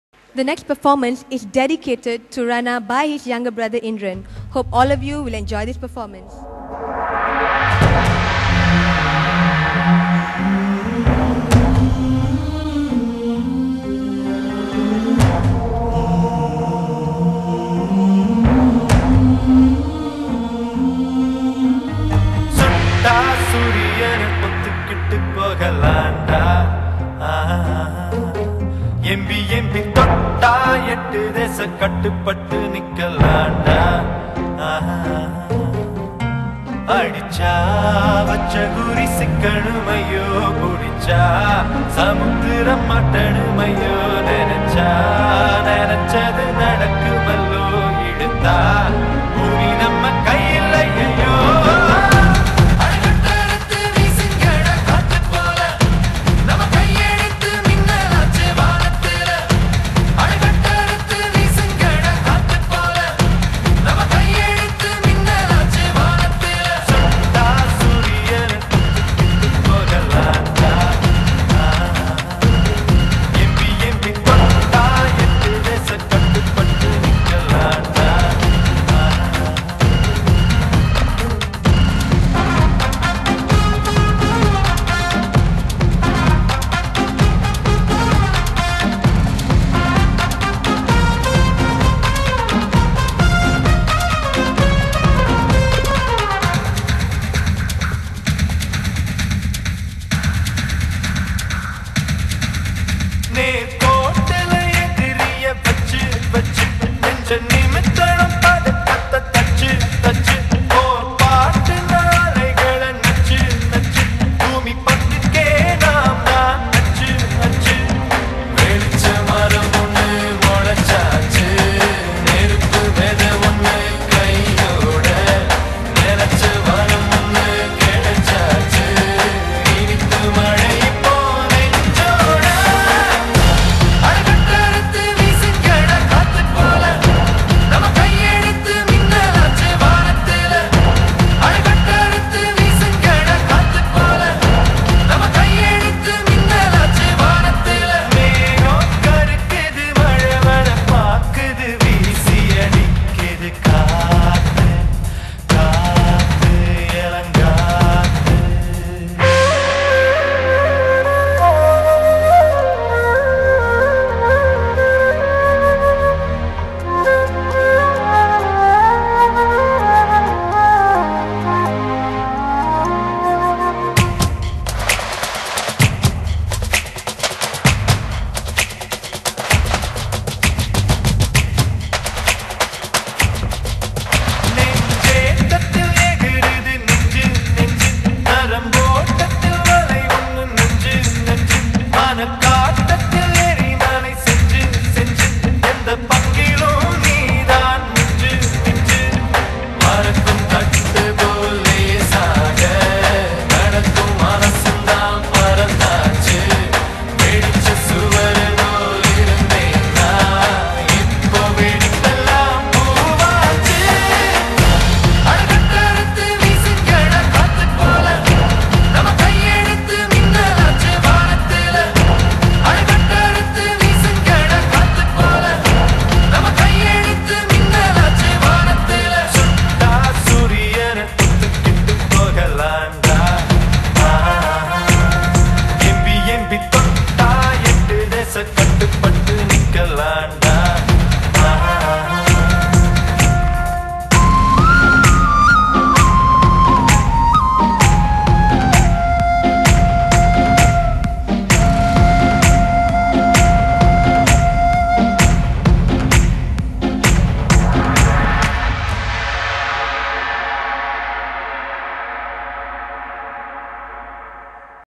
Indiran + Ranna (NO ENTRY) LIVE
(MOV Productions) - Shot & Edit By Rocky & Crew, Ranna (NO ENTRY) & Sunitha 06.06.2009 Wedding Dinner Night at Sunway Hotel Ballroom Ranna, Jack, Paneer, Jacob, MC Loga & More on Stage LIVE!!!